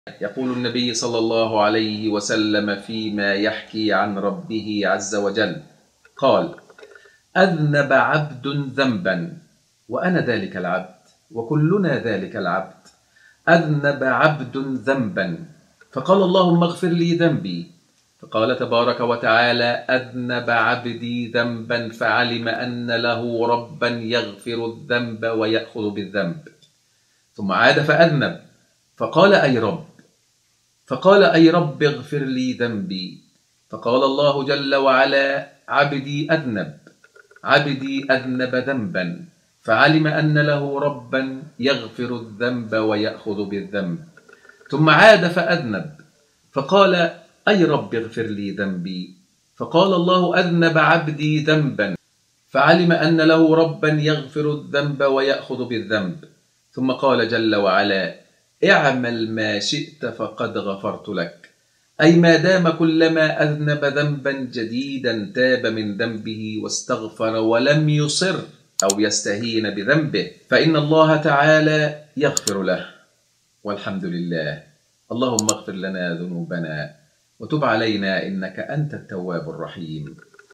أذنب عبد ذنبا | سعيد بدوي
يقول النبي صلى الله عليه وسلم فيما يَحْكِي عن رَبِّهِ عَزَّ وَجَلَّ، قالَ: أَذْنَبَ عَبْدٌ ذَنْبًا، وأنا ذلك العبد وكلنا ذلك العبد أَذْنَبَ عَبْدٌ ذَنْبًا فَقالَ: اللَّهُمَّ اغْفِرْ لي ذَنْبِي، فَقالَ تَبَارَكَ وَتَعَالَى: أَذْنَبَ عَبْدِي ذَنْبًا، فَعَلِمَ أنَّ له رَبًّا يَغْفِرُ الذَّنْبَ، وَيَأْخُذُ بالذَّنْبِ، ثُمَّ عَادَ فأذْنَبَ، فَقالَ: أَيْ رَبِّ اغْفِرْ لي ذَنْبِي، فَقالَ الله جل وعلا: عَبْدِي أَذْنَبَ ذَنْبًا، فَعَلِمَ أنَّ له رَبًّا يَغْفِرُ الذَّنْبَ، وَيَأْخُذُ بالذَّنْبِ، ثُمَّ عَادَ فأذْنَبَ فَقالَ: أَيْ رَبِّ اغْفِرْ لي ذَنْبِي، فَقالَ الله: أَذْنَبَ عَبْدِي ذَنْبًا، فَعَلِمَ أنَّ له رَبًّا يَغْفِرُ الذَّنْبَ، وَيَأْخُذُ بالذَّنْبِ، ثم قال جل وعلا ..اعْمَلْ ما شِئْتَ فقَدْ غَفَرْتُ لَكَ: أي ما دامَ كُلَّما أذنَبَ ذنبًا جَديدًا تابَ من ذنبِه واستَغفَرَ ولم يصر أو يستهين بذنوبه ، فإن الله تعالى يغفر له...والحمد لله اللهم اغفر لنا وتب علينا إنك أنت التواب الرحيم